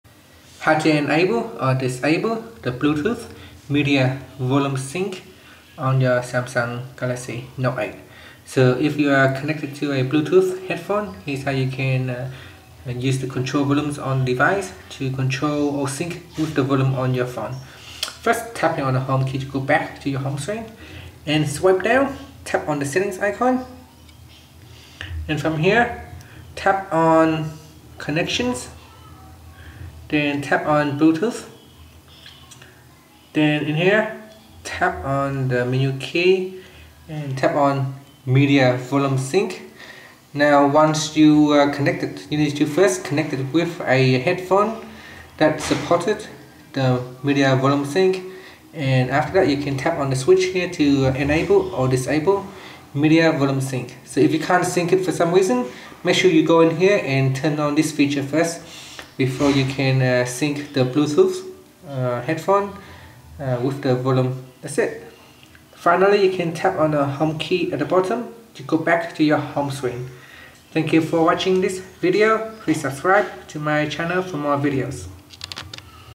Samsung Galaxy Note 8: How to Enable / Disable Bluetooth Media Volume Sync
Learn how you can enable or disable Bluetooth Media Volume Sync on Samsung Galaxy Note 8.